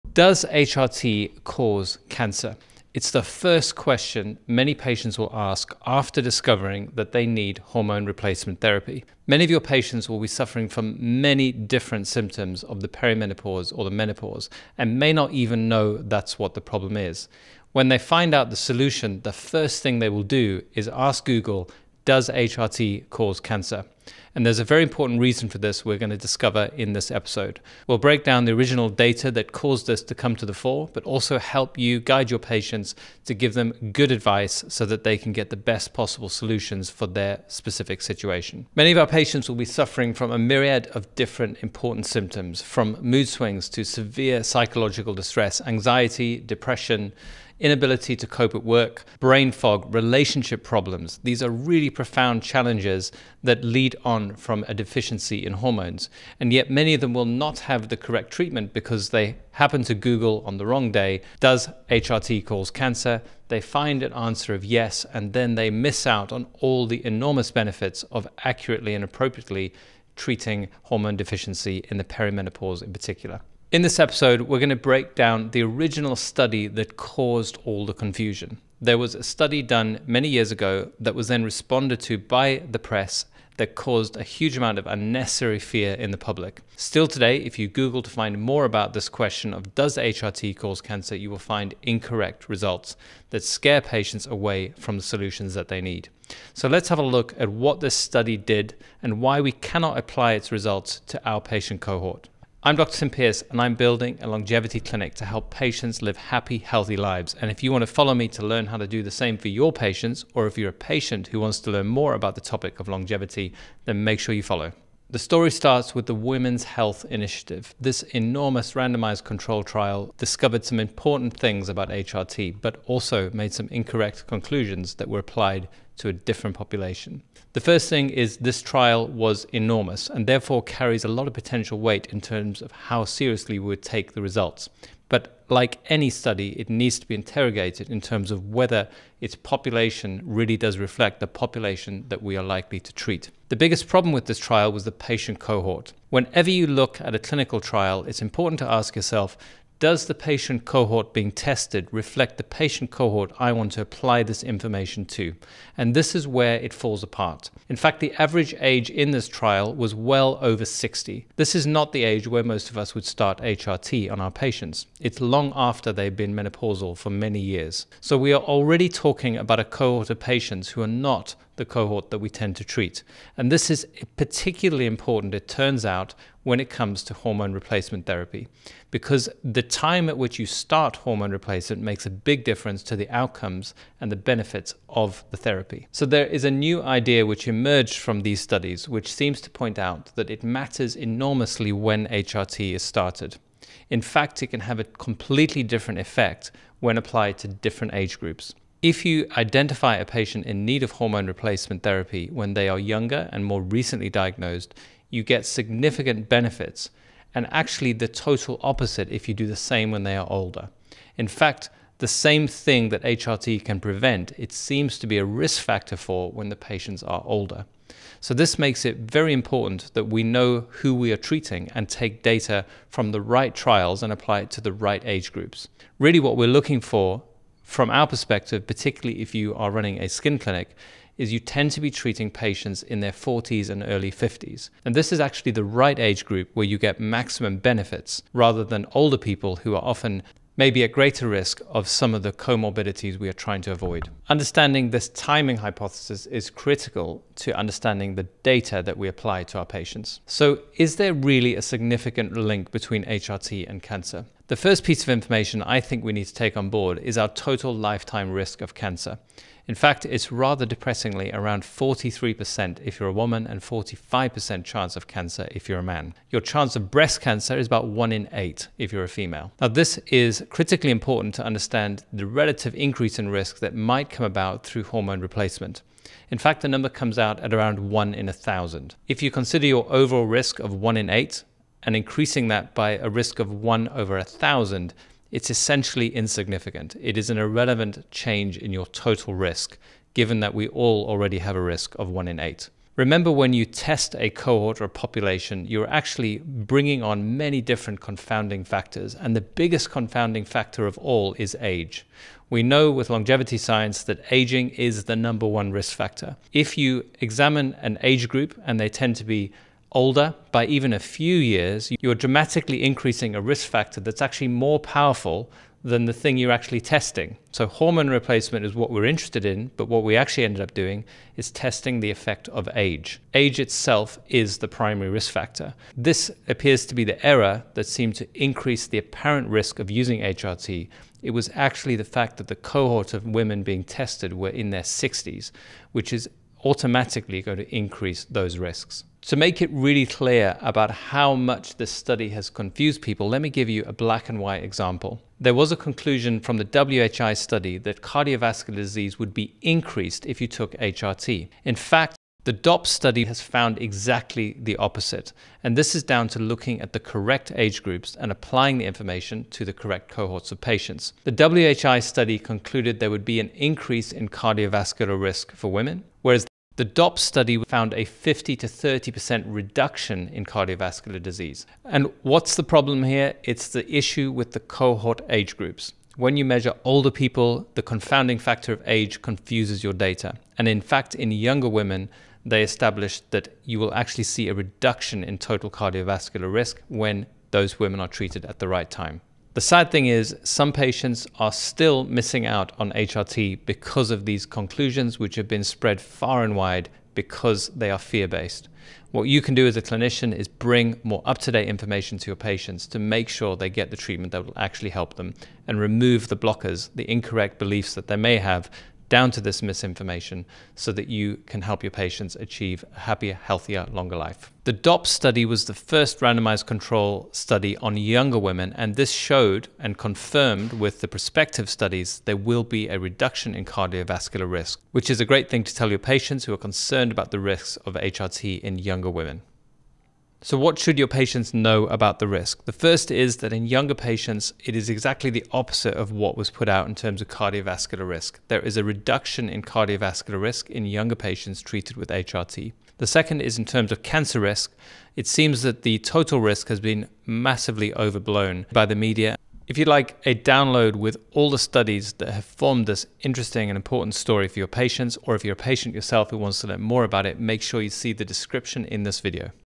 What they DIDN'T tell you about the HRT & Cancer Study
Does HRT cause cancer? It's often the first question patients ask when considering hormone replacement therapy. In this episode, we break down the science behind HRT and cancer risk, examining the famous Women's Health Initiative study that sparked widespread fear - and revealing crucial information that wasn't widely reported.

Key topics covered: • The real cancer risk numbers in context • Why the timing of starting HRT matters significantly • How age impacts study results and risk factors • The surprising cardiovascular benefits for younger women • What clinicians should tell their patients about HRT risks

Whether you're a healthcare professional advising patients or someone considering HRT yourself, this episode provides evidence-based clarity on this important topic.

🔍 Want to dive deeper? Download our free guide with detailed study breakdowns and risk assessments

Follow me as we build a longevity clinic in Cheshire and train clinicians worldwide to help their patients live longer, healthier lives.